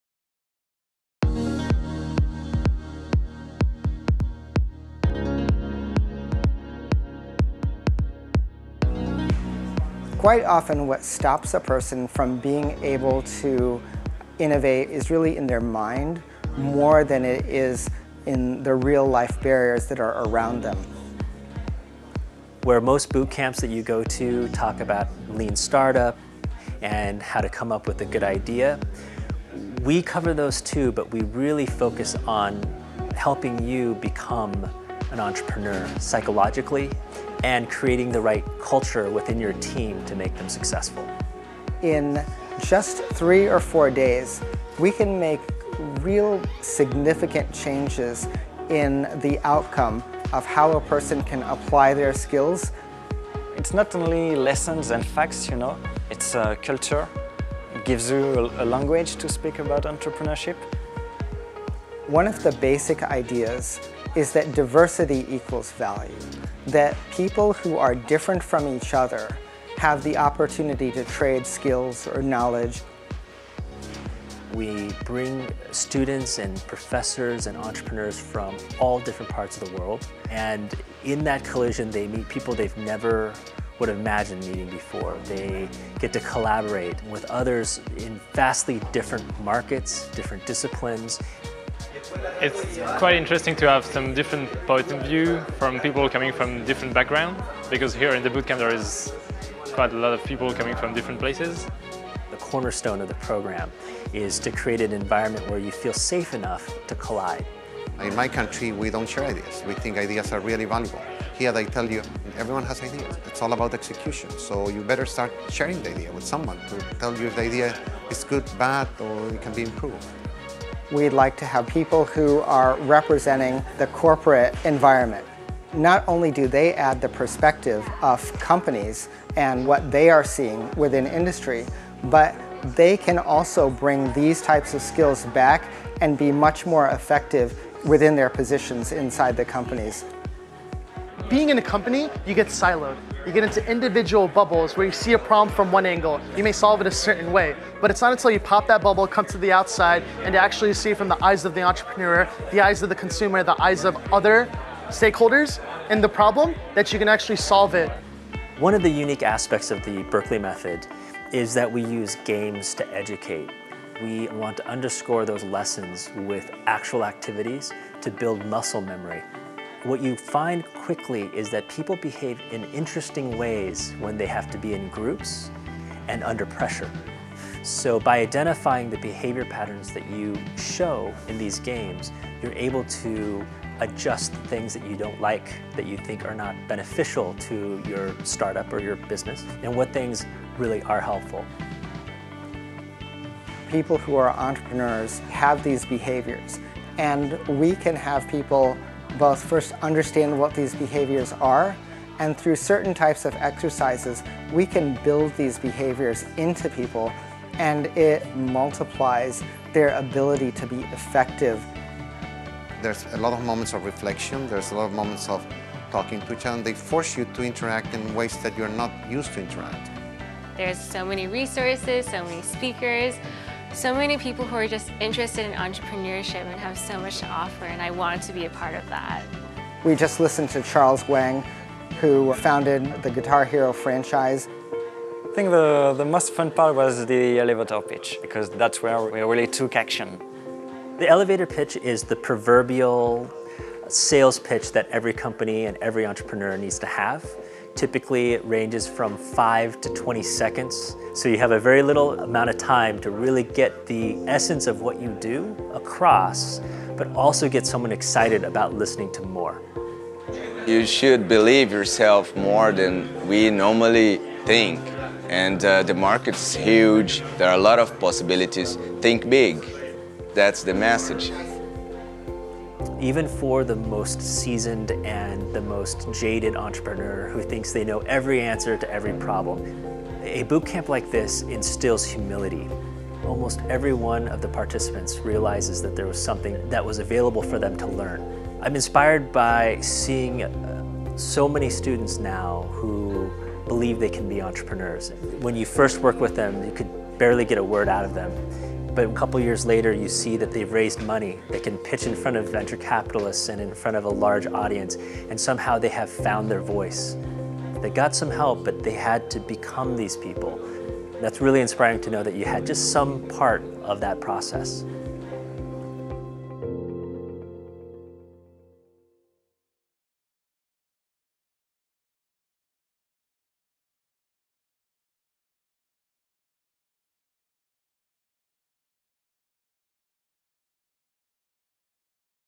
Berkeley Method of Entrepreneurship - SCET
The Pantas and Ting Sutardja Center for Entrepreneurship & Technology hosts the bi-annual Berkeley Method of Entrepreneurship Bootcamp (BMoE), a week venture workshop. The bootcamp facilitates immersive learning for new venture creation. This workshop offers a one-of-a-kind experience in one of the premier institutions in Silicon Valley. During the program, participants will attend sessions that weave together lectures and interactive game-based exercises, receive one-on-one mentoring from Sutardja Center faculty and industry experts; and learn in a collaborative environment.
To learn more, visit scet.berkeley.edu/bmoe-spring-2017/